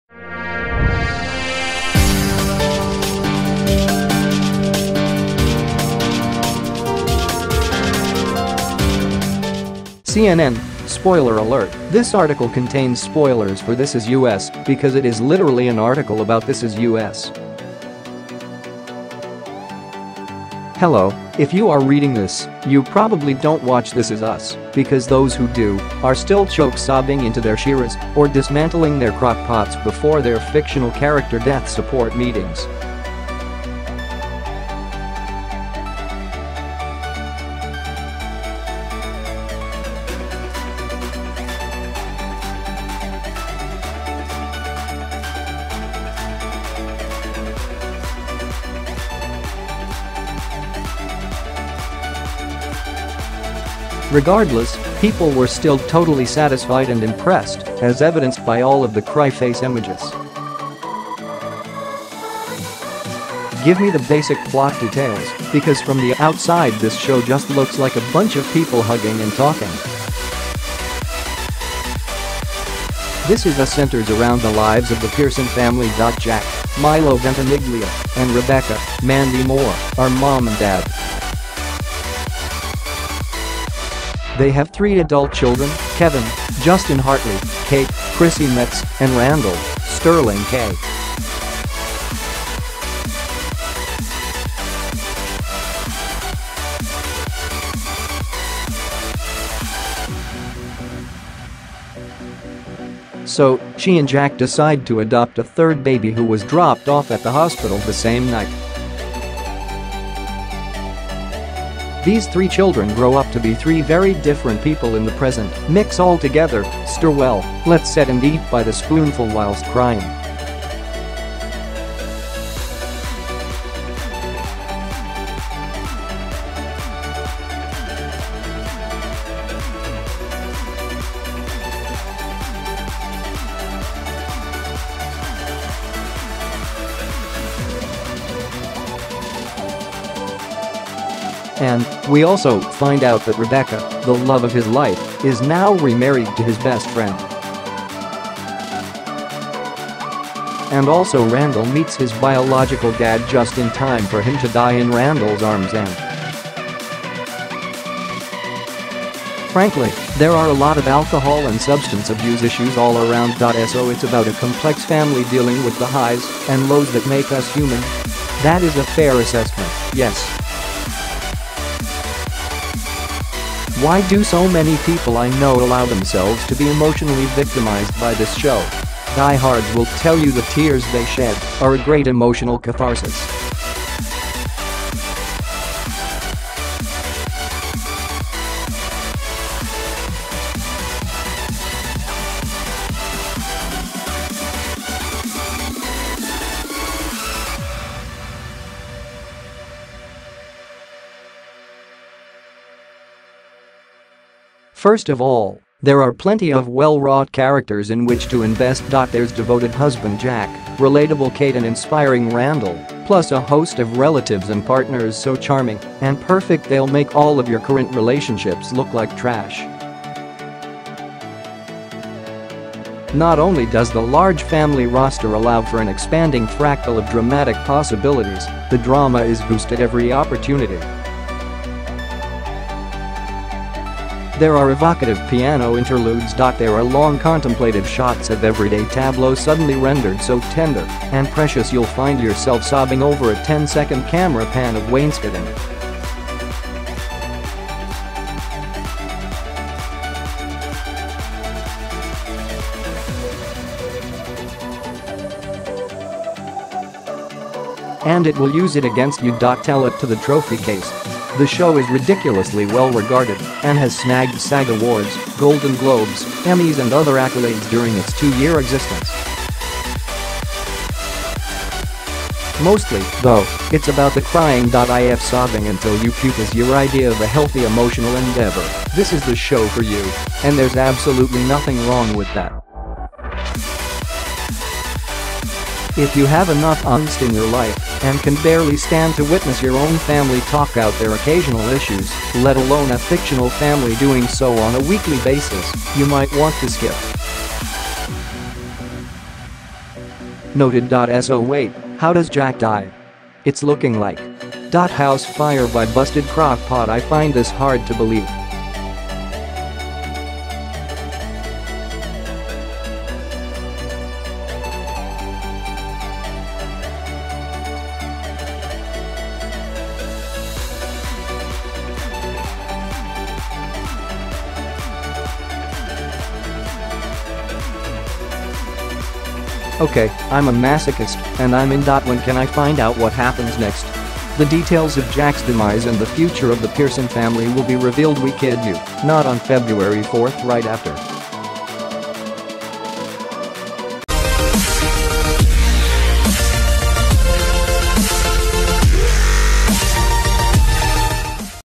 All the questions you were afraid to ask about the show
(CNN) SPOILER ALERT: THIS ARTICLE CONTAINS SPOILERS FOR "THIS IS US" BECAUSE IT IS LITERALLY AN ARTICLE ABOUT "THIS IS US." Hello, if you are reading this you probably don't watch "This is Us," because those who do are still choke-sobbing into their shiraz or dismantling their Crock-Pots before their fictional-character-death support meetings.Allow us to explain everything.Why is everyone talking about "This is Us" right now?  The beloved NBC family drama isn't even through its second season, but it feels like every week brings another emotional climax or character birth/death/breakdown/revelation/triumph so wrenching or life-affirming all of your social media feeds immediately devolve into a series of cry-face emojis.But one of the biggest moments happened this week when viewers found out how one of the main characters may have died , even though they KNEW he would die at some point for about 20 episodes now, and all of the clues to how he'd probably die were introduced quite clearly a few episodes back.Regardless, people were still totally satisfied and impressed, as evidenced by all of the cry-face emojis.Give me the basic plot details, because from the outside this show just looks like a bunch of people hugging and talking."This Is Us" centers around the lives of the Pearson family.Jack (Milo Ventimiglia) and Rebecca (Mandy Moore) are mom and dad.They have three adult children: Kevin (Justin Hartley), Kate (Chrissy Metz) and Randall (Sterling K.Brown).Take a deep breath, because it gets real, fast:  In 1980, Rebecca was pregnant with triplets but one of the babies died.So, she and Jack decide to adopt a third baby who was dropped off at the hospital the same night.These three children grow up to be three very different people in the present:  Mix all together, stir well, let set and eat by the spoonful whilst crying.It doesn't stop there! The show bounces back and forth in time, using younger versions of Jack, Rebecca and company to explore the main couple's love story as well as the small threads of early-life trauma that eventually become giant rat's nests of issues for their children.Oh, and it is by this convenient structure that we learn that dad Jack -- at some point when his kids were teens -- died.AND, we also find out that Rebecca, the love of his life, is now remarried to his best friend.and also Randall meets his biological dad just in time for him to die in Randall's arms and.frankly, there are a lot of alcohol and substance abuse issues all around.So it's about a complex family dealing with the highs and lows that make us human?  That is a fair assessment, yes.Why do so many people I know allow themselves to be emotionally victimized by this show?  Die-hards will tell you the tears they shed are a great emotional catharsis.Plus, the constant roller coaster of bad things (Dead dads! Miscarriages! Regrets!) and good things (Family! Good husbands! The Super Bowl!) makes it hard to let go once you've hopped on.I'm still having troubl